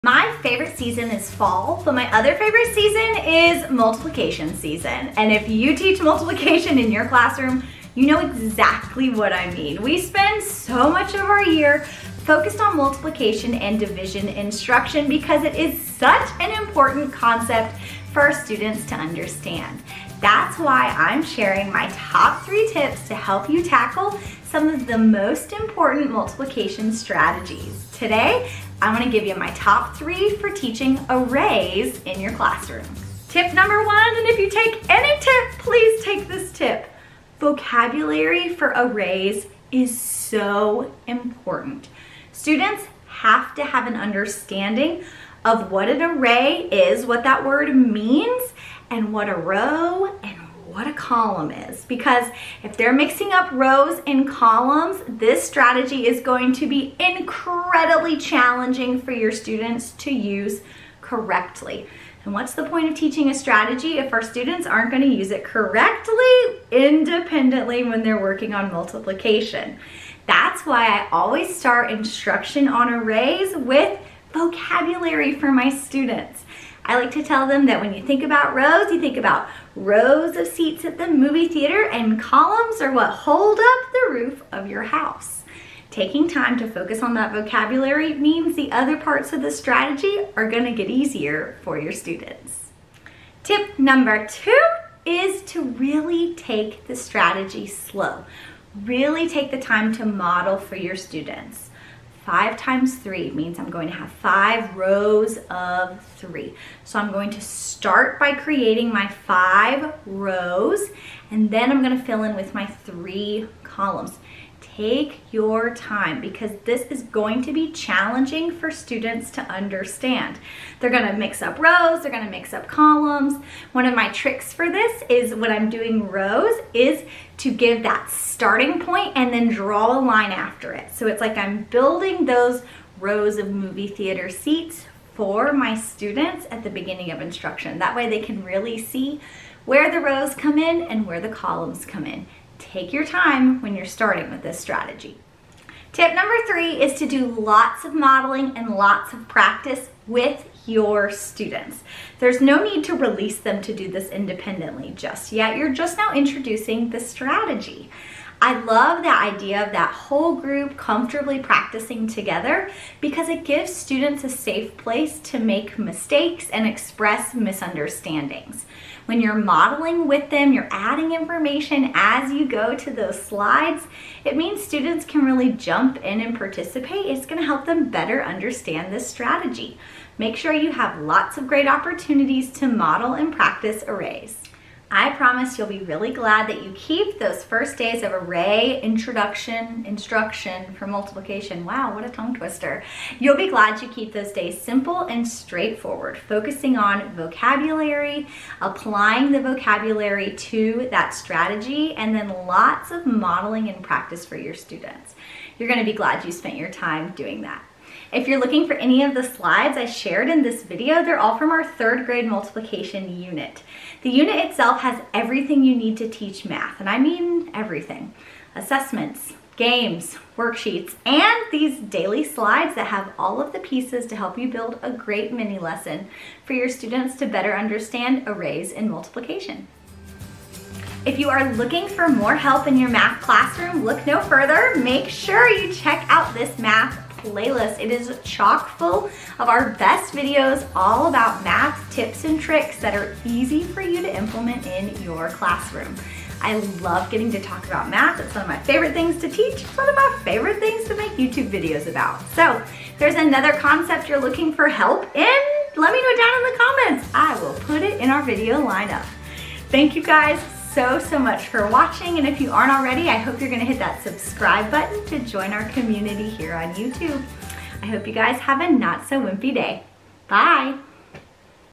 Top 3 Tips to Help You Teach Arrays!
Building Arrays is a Multiplication Must-Teach! Today I am sharing my top three instructional tips to help you tackle this in your math classroom!